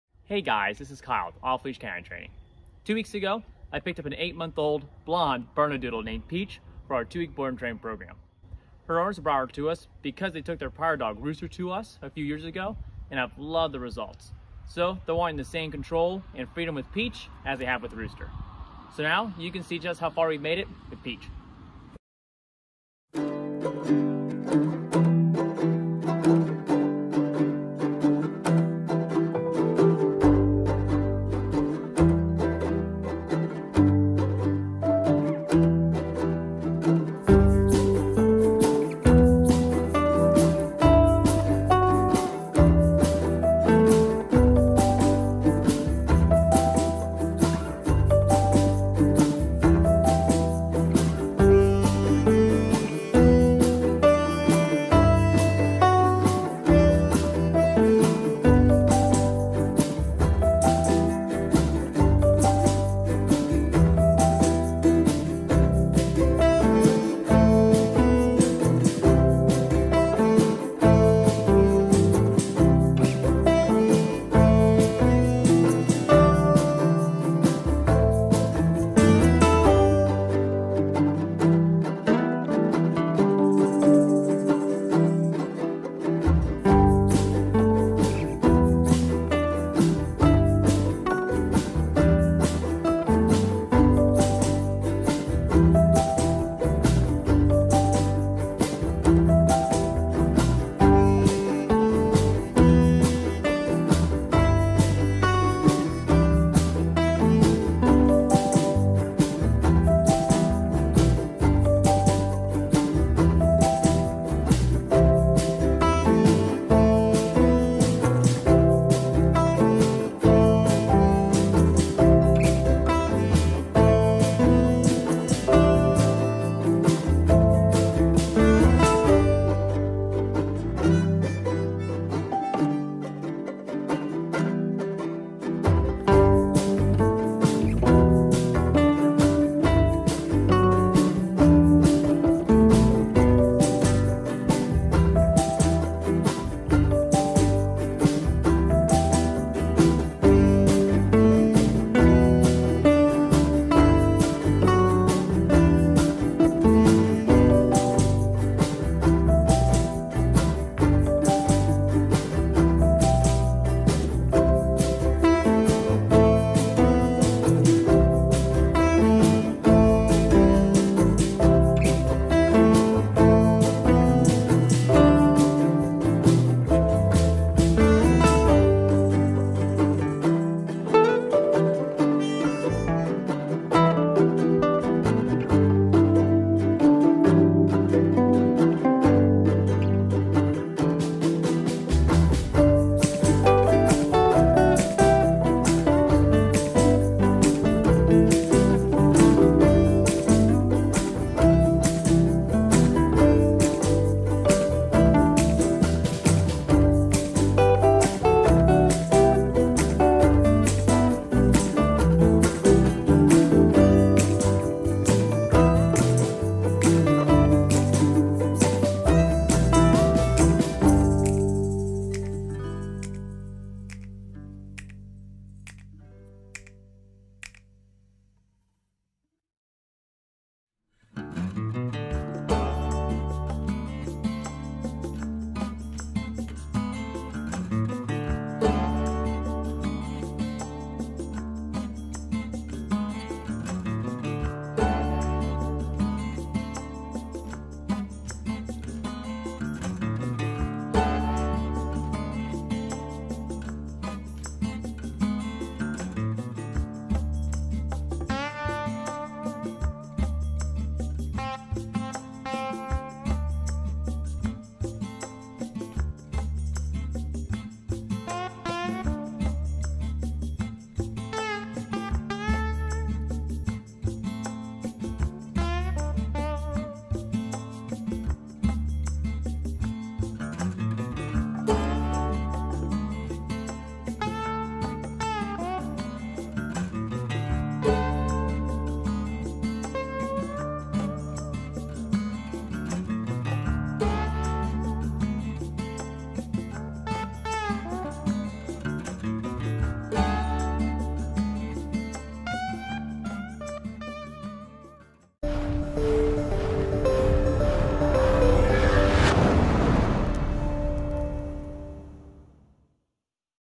Peach | 8 MO Bernedoodle | Anxiety Training | E Collar Training
Take a look at the progress we were able to make with Peach in just 2 weeks!